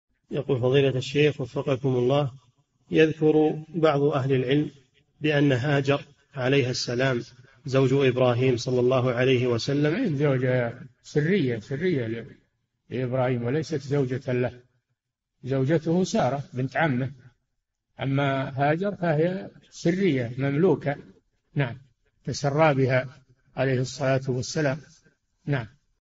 [3084- 4717] هل هاجر عليها السلام زوجة إبراهيم عليه السلام؟ - الشيخ صالح الفوزان
[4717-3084] يذكر بعض أهل العلم بأنّ هاجر عليها السلام زوج إبراهيم ﷺ؟
الجَوَابُ: ما هي زوجة، سرّية لإبراهيم وليست زوجة له، زوجته سارة بنت عمه، أما هاجر فهي سرّية مملوكة، تسرى بها عليه الصلاة والسلام.
السيرة الذاتية :
للاشتراك في قناة الشيخ صالح الفوزان
لمشاهدة حلقات فتاوى على الهواء كاملة

للاستفسار والإقتراحات والدعم
رئيس الفريق
لمشروع كبار العلماء